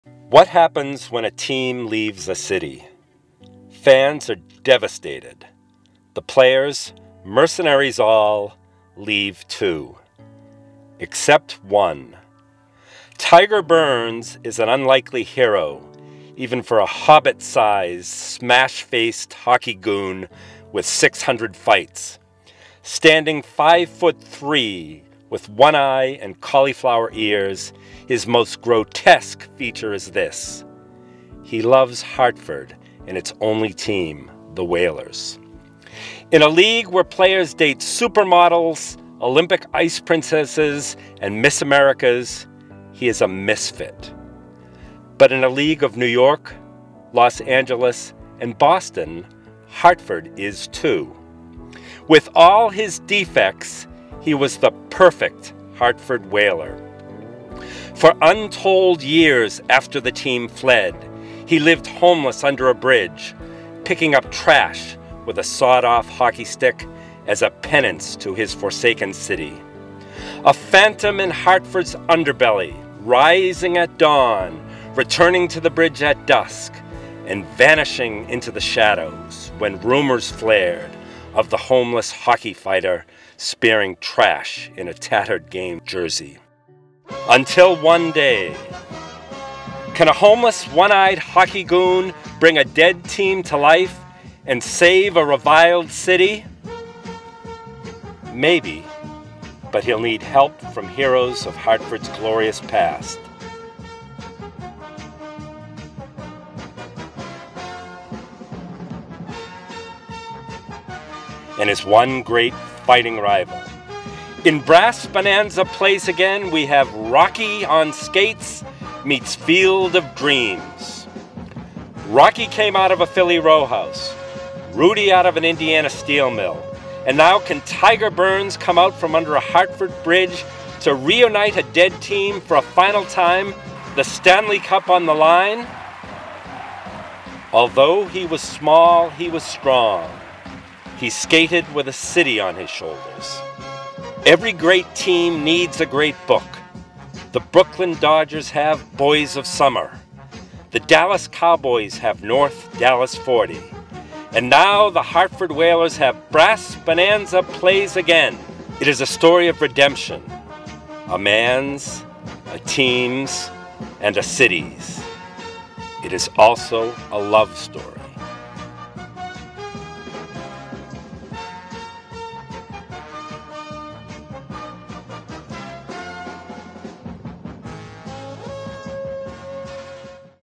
Brass Bonanza Plays Again - Hartford Whalers Book Trailer
Tragicomic story of Tiger Burns, one-eyed hockey goon who loves Hartford and its lowly team the Whalers. When the team flees to Carolina, he lives homeless under a bridge for years, spearing trash with a sawed-off hockey stick as penance. Can a hobbit-sized, smash-faced fighter reunite a dead team for a shot at the Stanley Cup--and save a reviled city? In Brass Bonanza Plays Again, we have "Rocky" meets "Field of Dreams." It is also a love story. A new novel by Robert Muldoon.